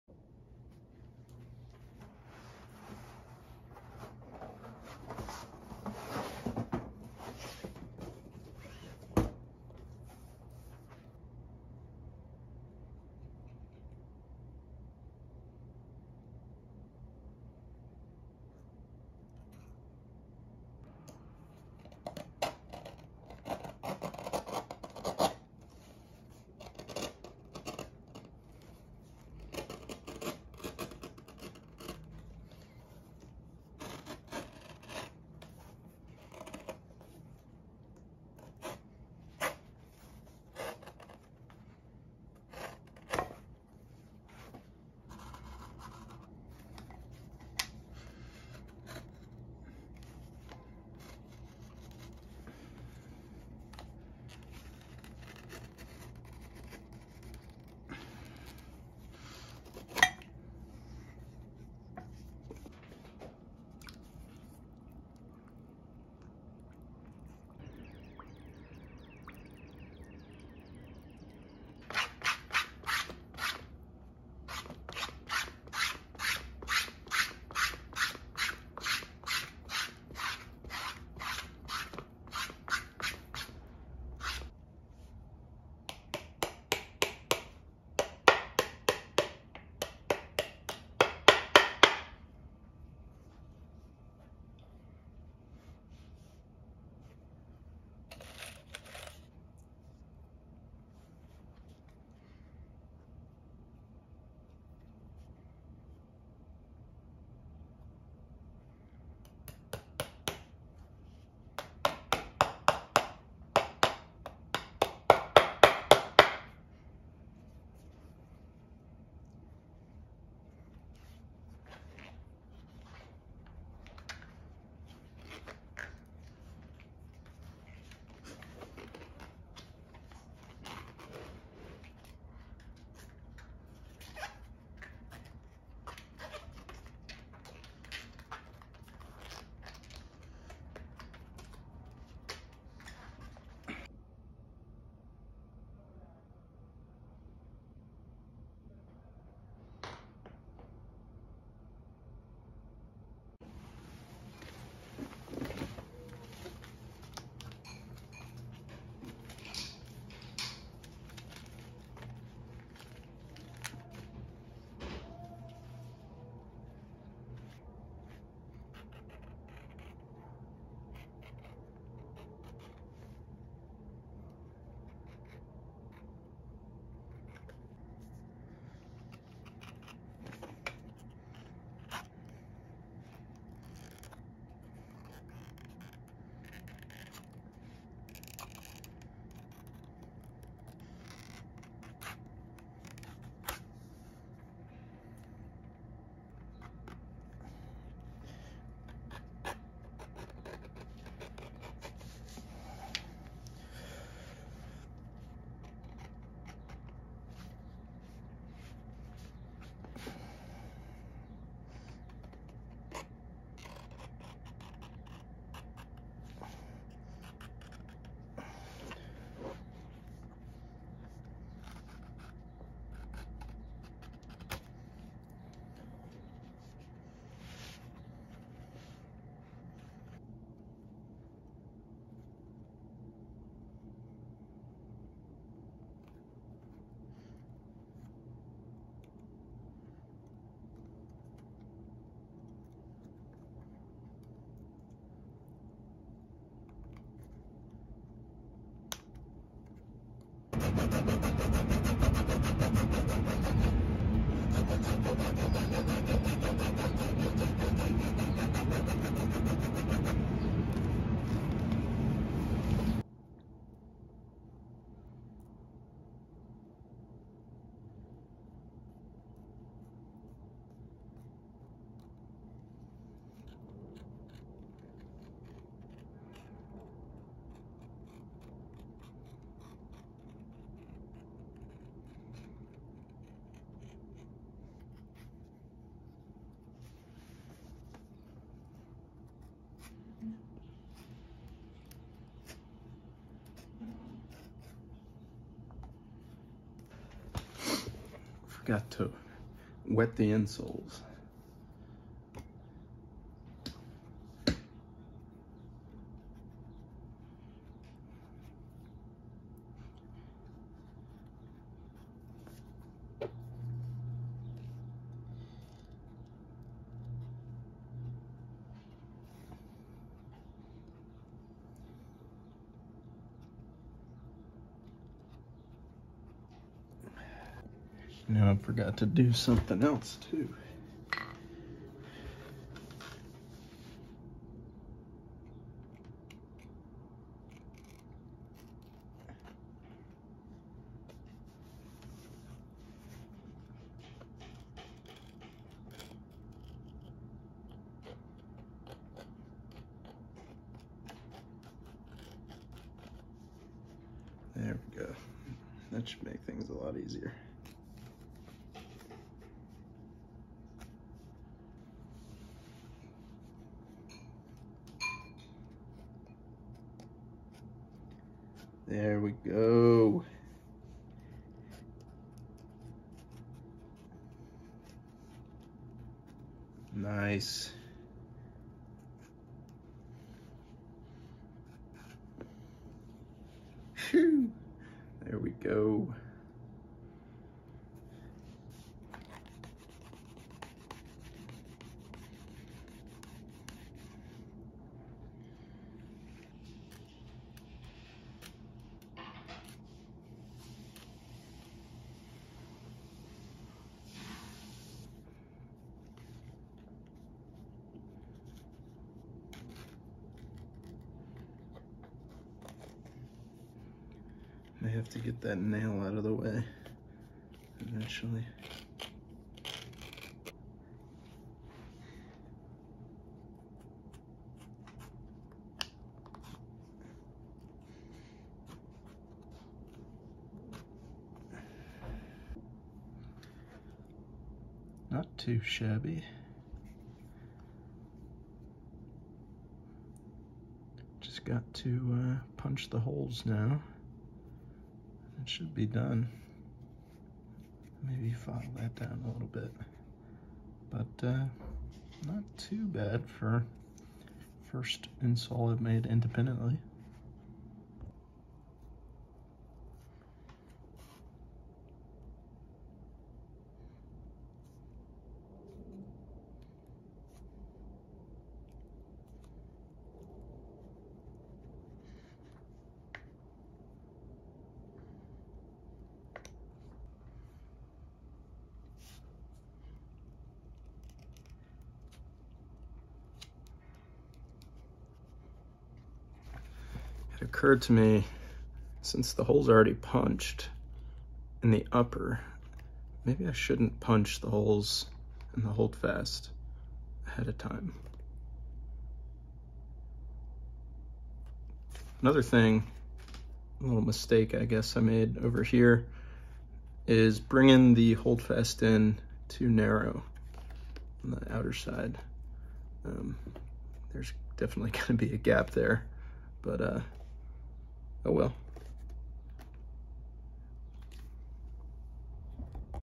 Resizing Old Shoes (Part 1 - Insole Prep) - Traditional Bespoke Shoemaking
Prepping insoles for handwelting.